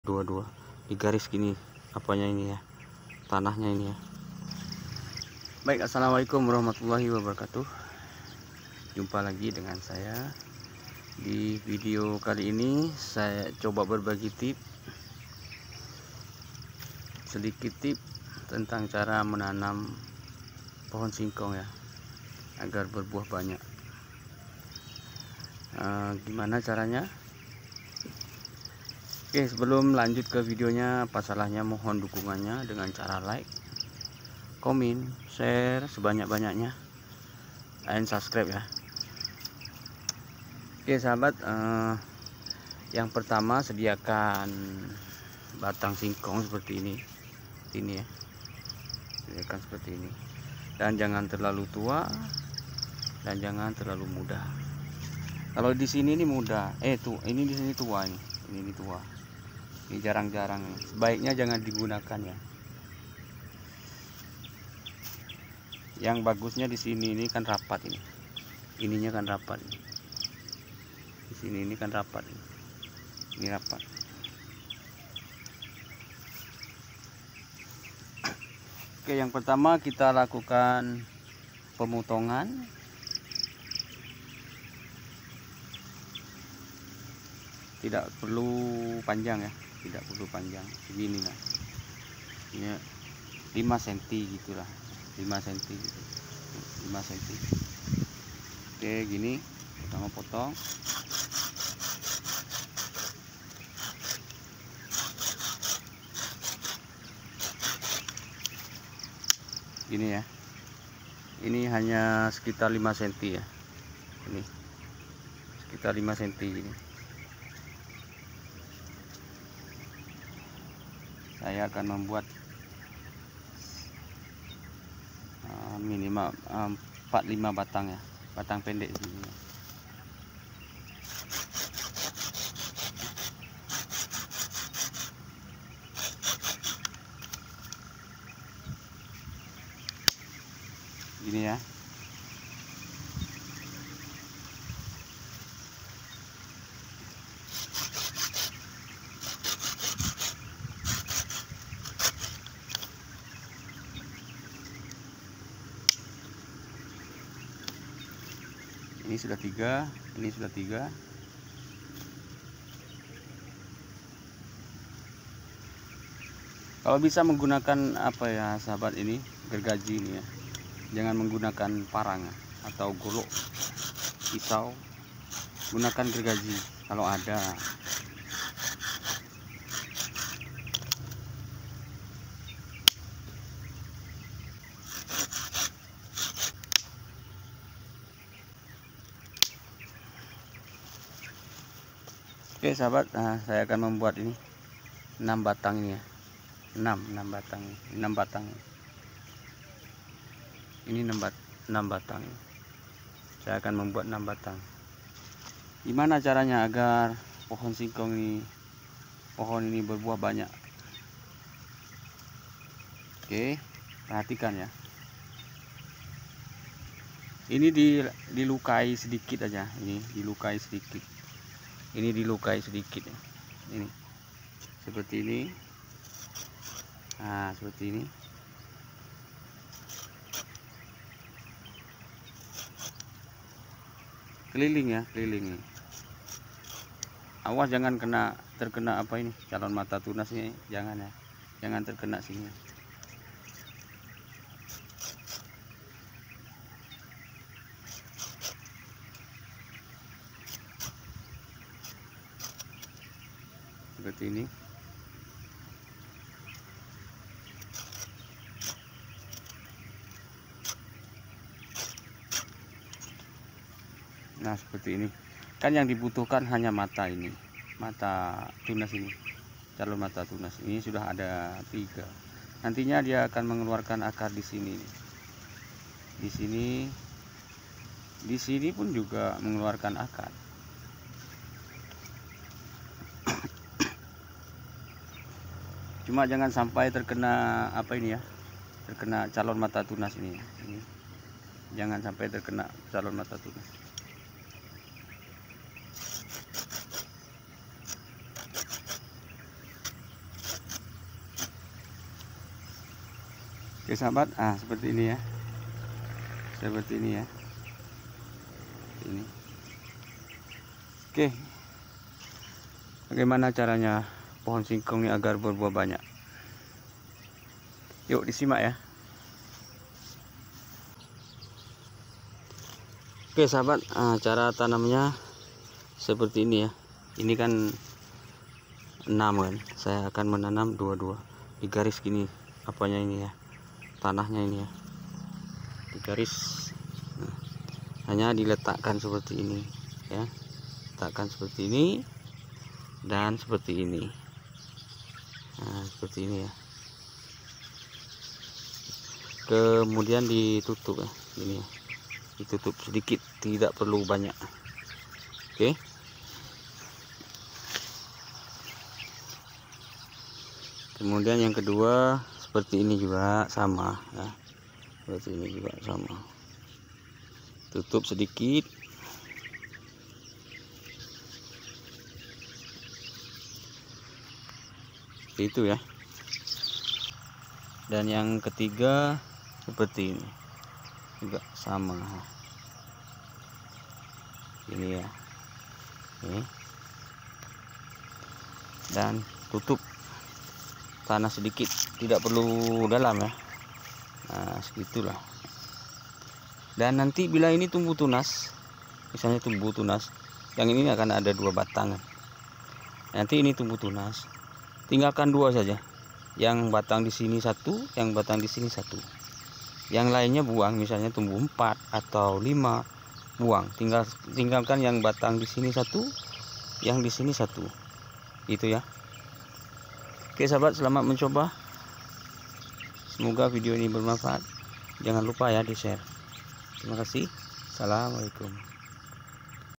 Penemuan terbaru cara tanam singkong berbuah banyak dengan teknik terbaru ini
video ini di buat sebagai media pembelajaran dalam bertani dan berkebun

singkong yang kita tanam dari jenis singkong yang bisa di konsumsi oleh semua orang

tidak membutuh kan lahan yang luas tanam di tepi rumah pun juga bisa